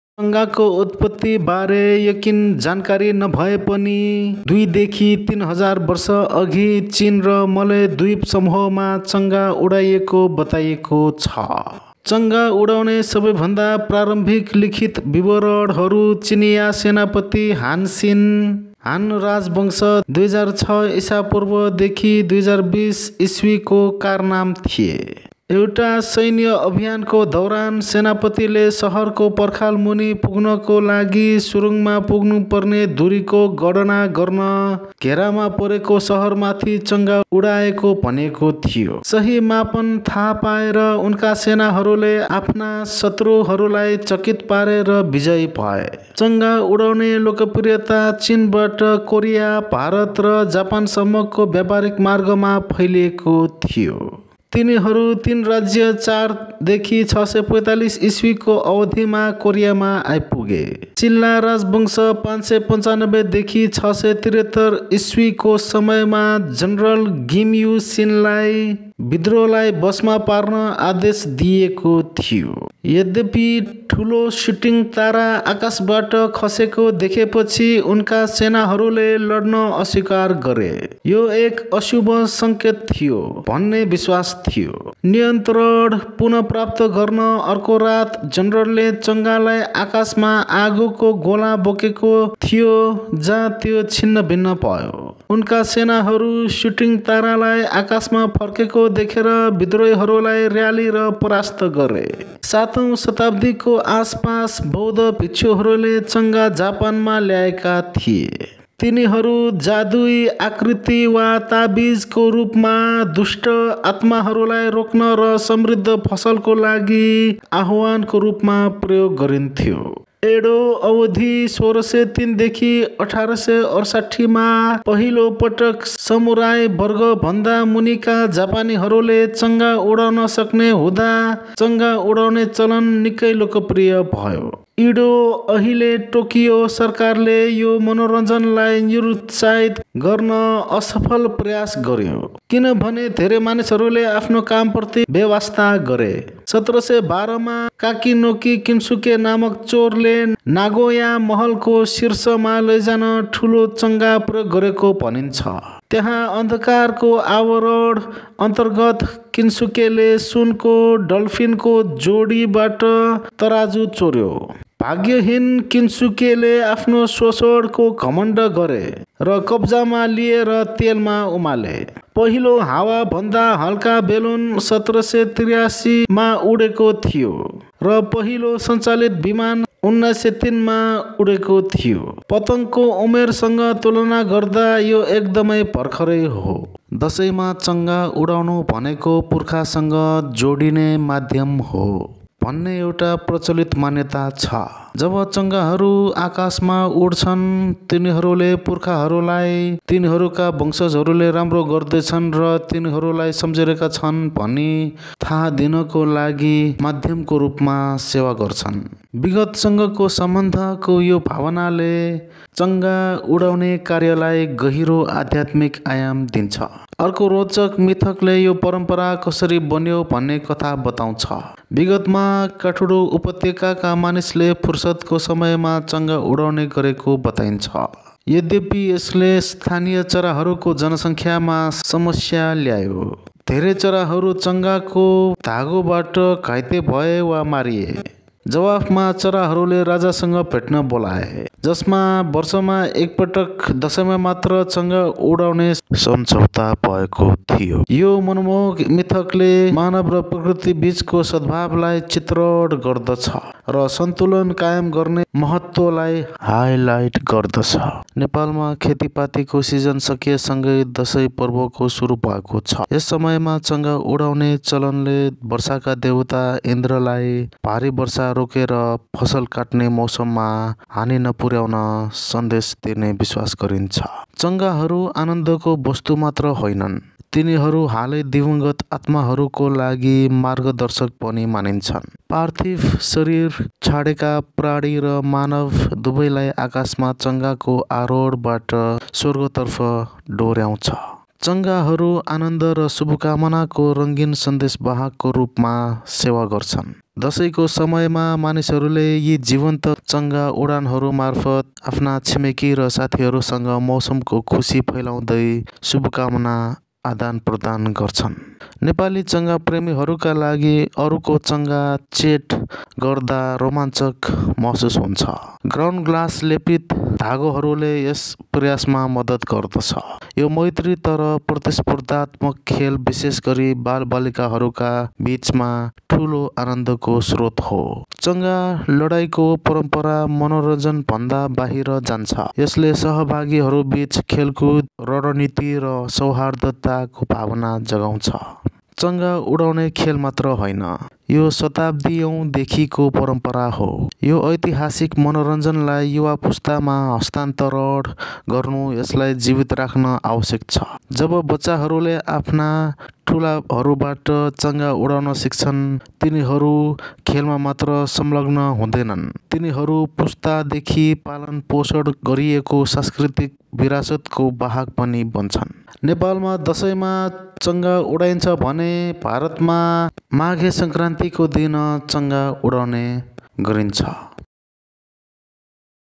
किन चङ्गा दशैंमा मात्र उडाइन्छ || Kite History || Kite Flying In Nepal
Connect With Nepal Dabali -
FacebookPage -

Business Queries -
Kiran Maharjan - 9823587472